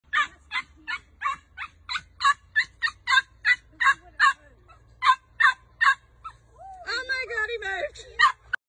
Deadly Red-bellied Snake on Dog Walk 😬🐍😱
Look what we saw on our dog walk this morning... a red-bellied black snake, yep, the poisonous ones. We thought it might be dead because it was super-still, but then it moved! Arrgghhh!
As you can hear, Charlie Frank tried to bark him away, which kinda worked as he slithered away to let us pass.
Needless to say, this was a very eventful dog walk.
Love your furry Australian friends...
Ruby-Alice
Rocky Bradman
Lady Lily-Grace
Charlie Frank